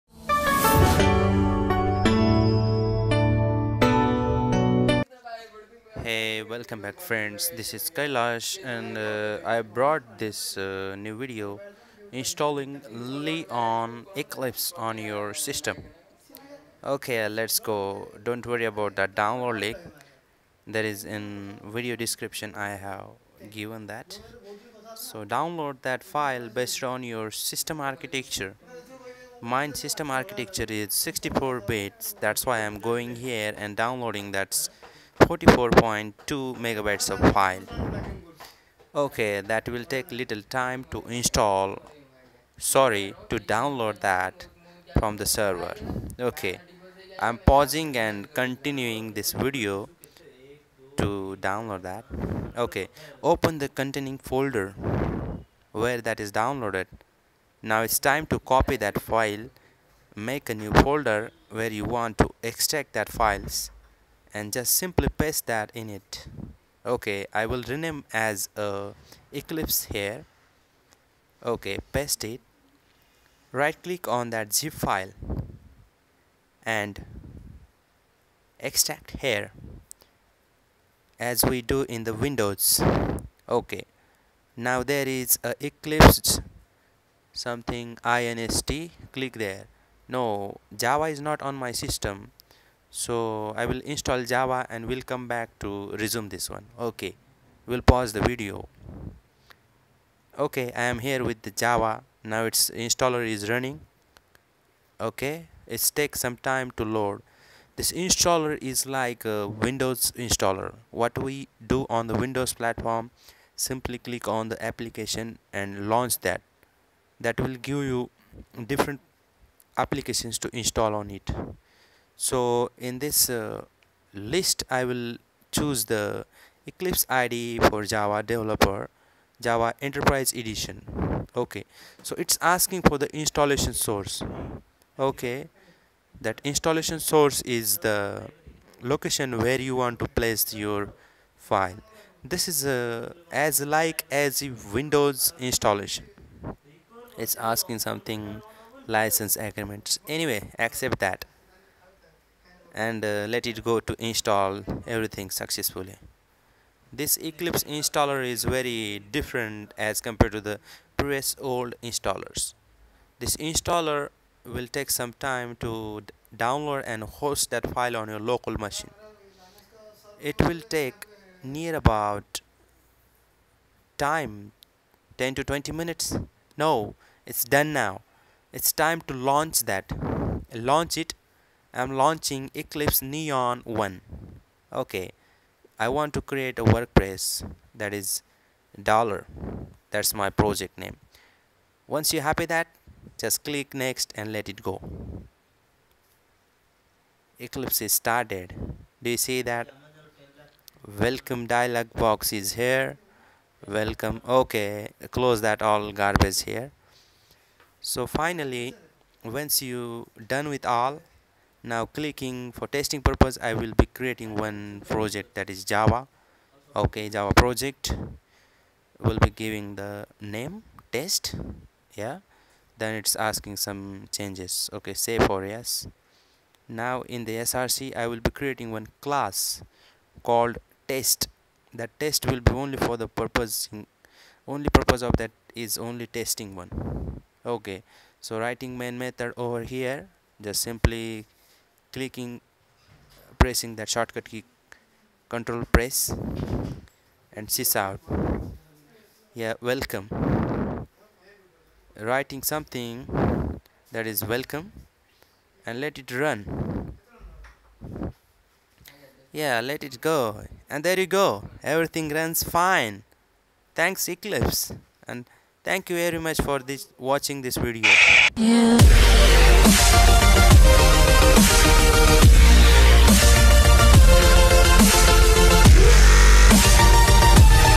Installing Eclipse Neon on Linux Ubuntu 16.04(Kubuntu, Lubunt, Xubuntu, Mint)
Download the installer here

Extract the archive, e.g the 64-bit version

tar xf eclipse-inst-linux64.tar.gz

Change your folder

How TO Install Java ON LINUX.

cd eclipse-installer

Run the installer WITHOUT sudo to UPGRADE the installer

    Note the yellow exclamation mark in the top right corner.